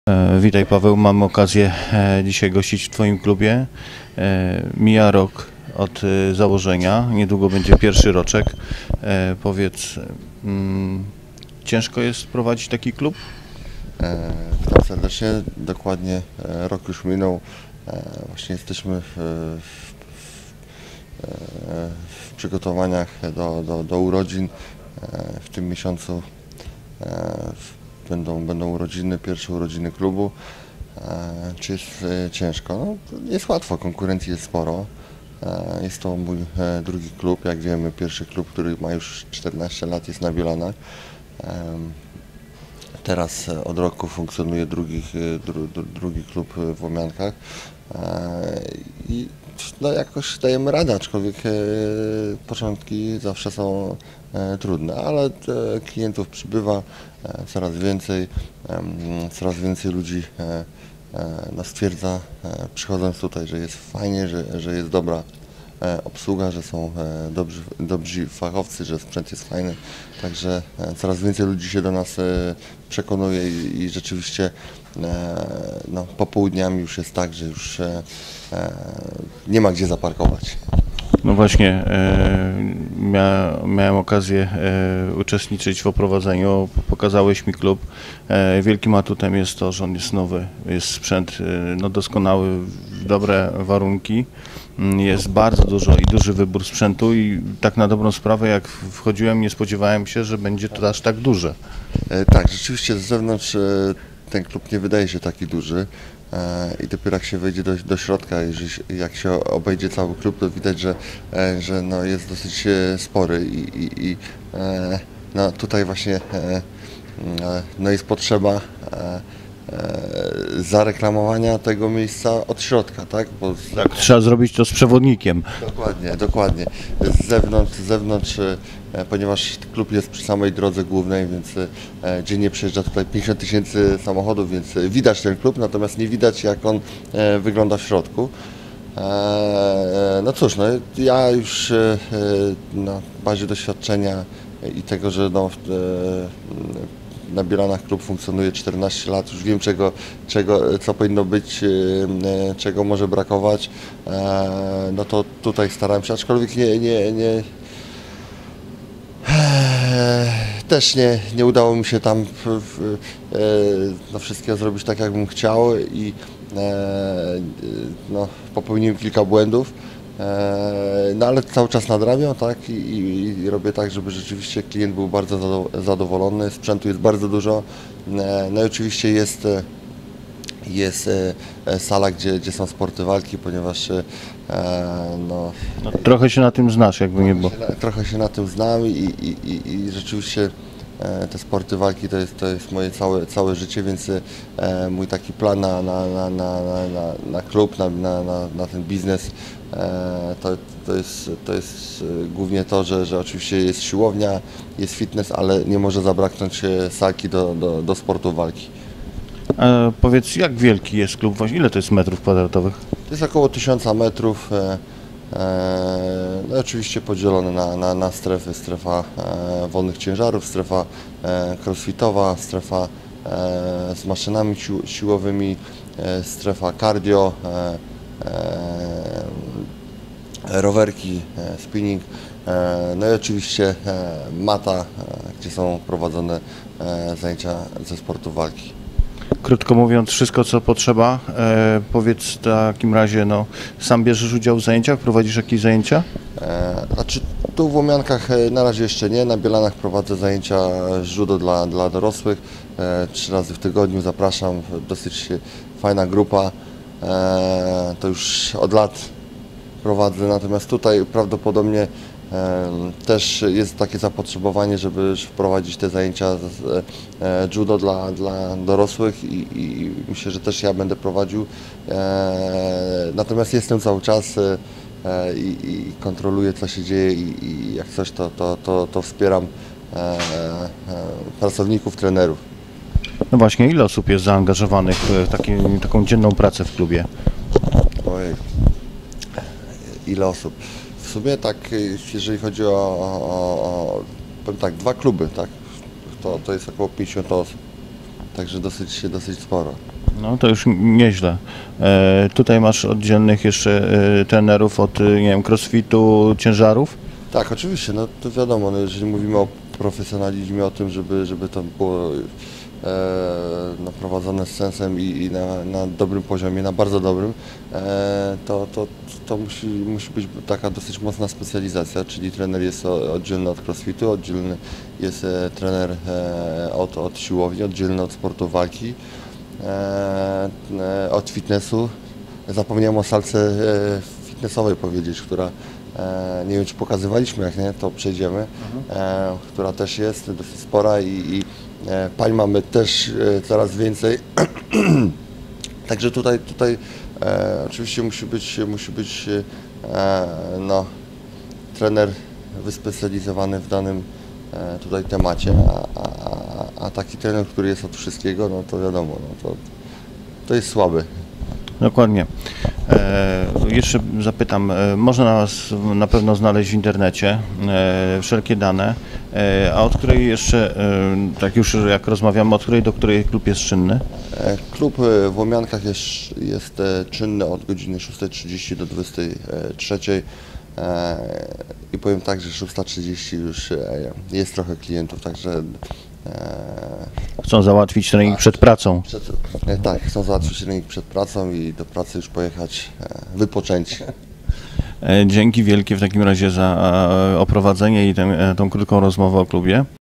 Paweł Nastula o klubie w Łomiankach
- Portal o wszystkich sportach walki przedstawia klub wywiad na temat Nastula Club Łomianki, prowadzony przez legendarnego polskiego judokę, Pawła Nastulę.